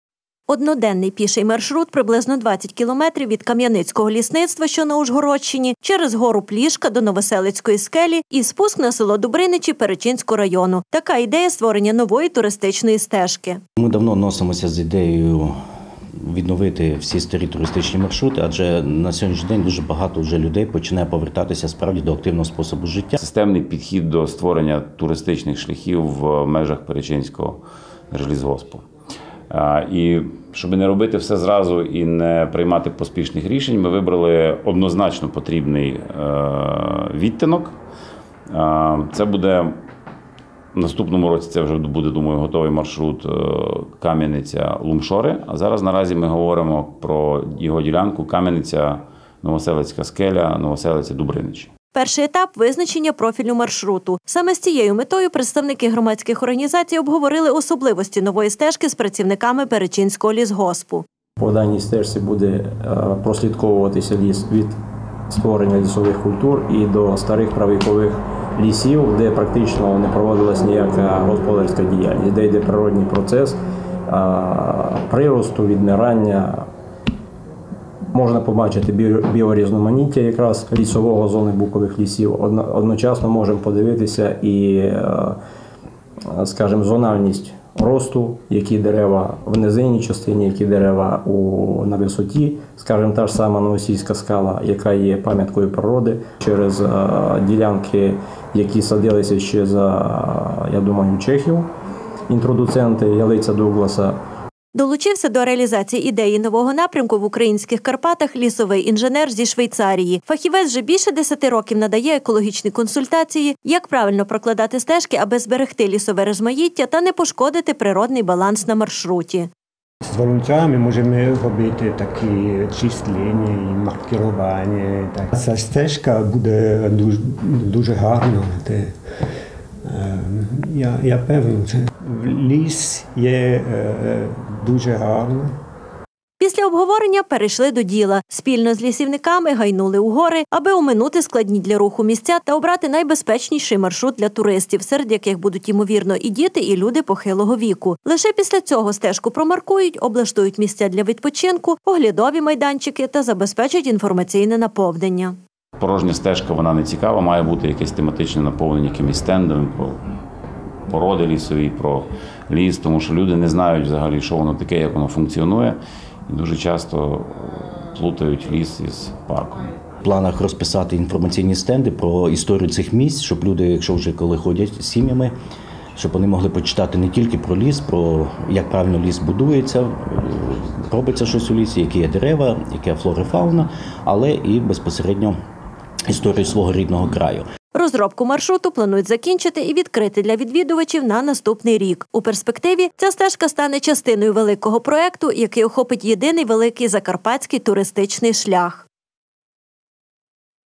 Новий маршрут. Туристичні стежки Закарпаття
Новий піший туристичний маршрут прокладуть на Перечинщині. Стежка буде з’єднувати Перечинський та Ужгордський райони, а згодом планують її продовжити аж до Великоберезнянщини. Цей напрям розробили представники громадських організацій «Туристичне товариство «Карпатські стежки» та «Закарпатська регіональна організація розвитку туризму», спільно з лісівниками Перечинщини та Швейцарії.